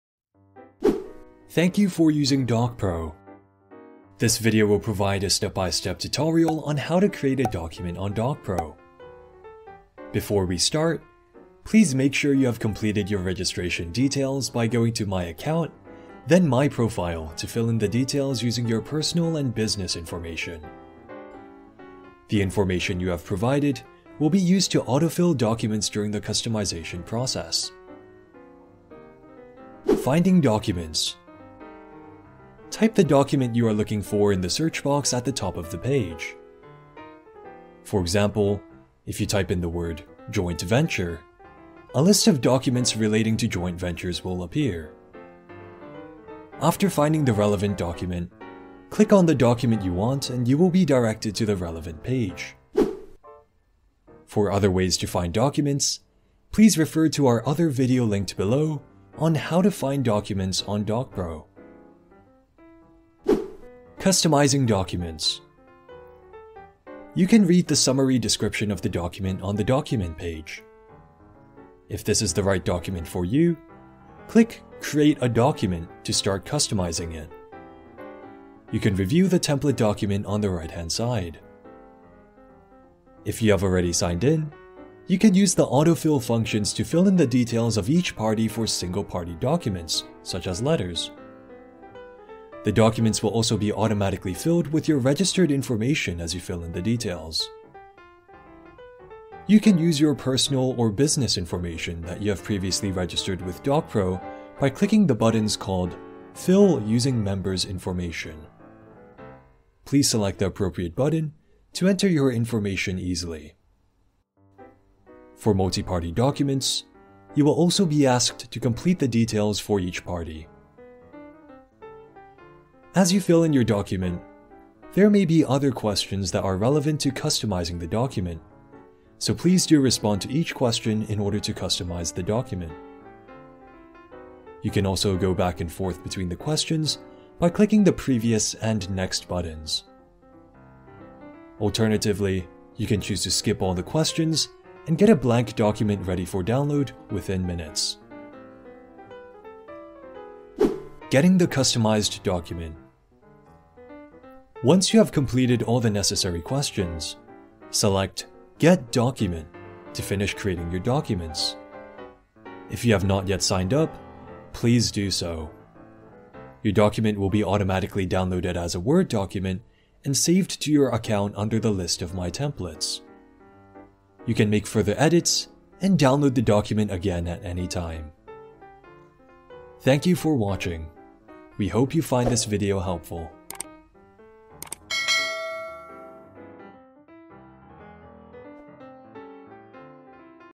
DocPro - How to Create Documents
Finding and creating documents on DocPro has never been easier. This is our second FAQ video explaining how to create your own documents on DocPro.